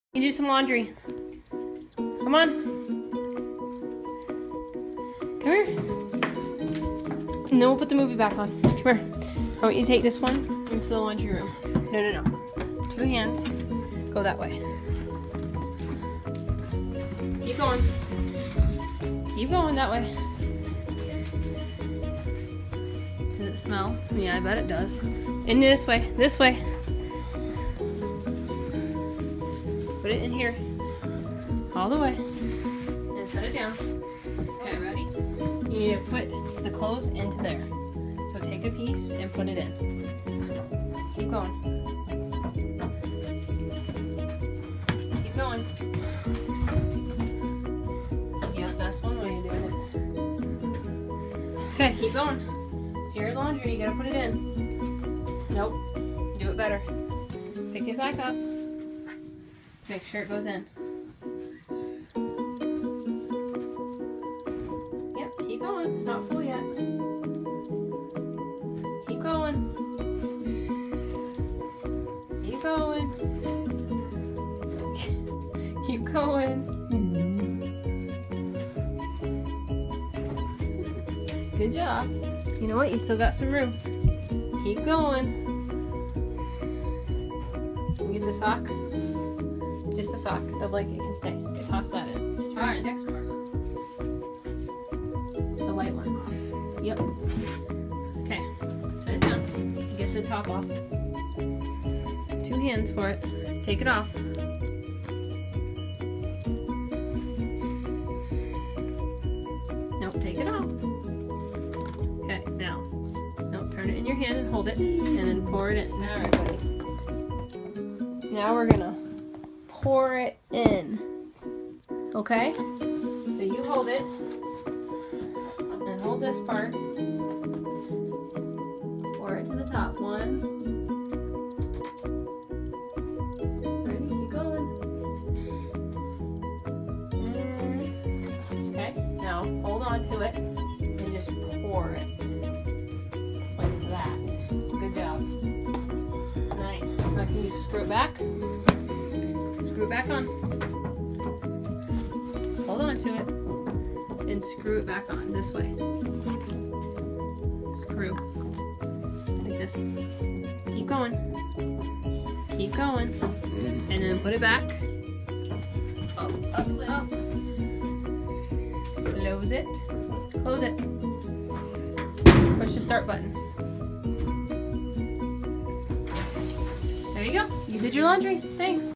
Teen with autism self help skills: doing laundry!
First time doing laundry! Will use visuals next!